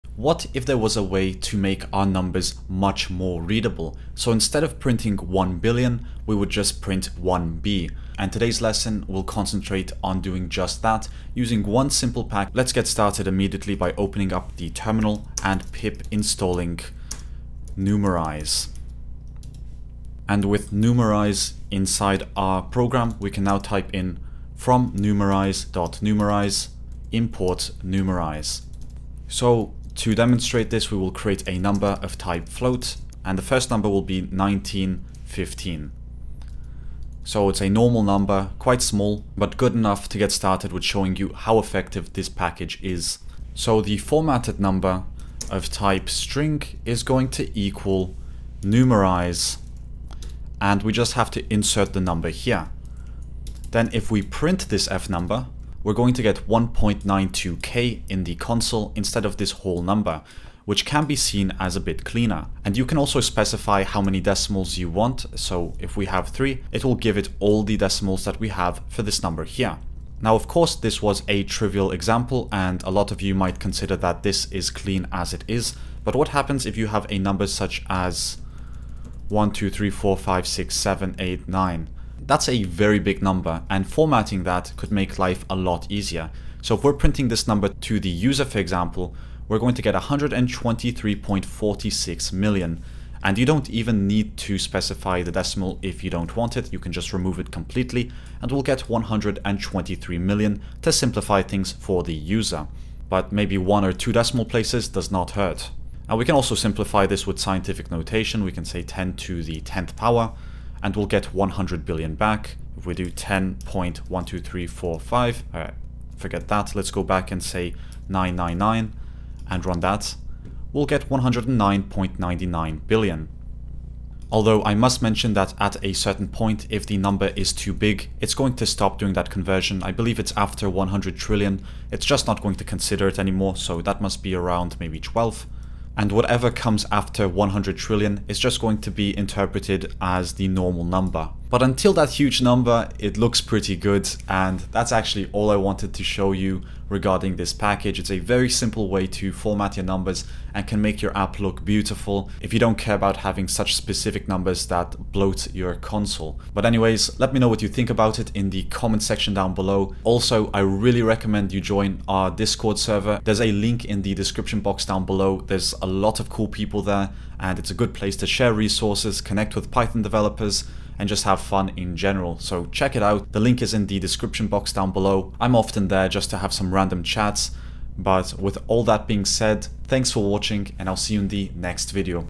THIS Is An Even CLEANER Way To FORMAT Numbers In Python!
Here's an even cleaner way to format your numbers in Python! It takes advantage of the numerize package, which converts any number into something that's actually human readable!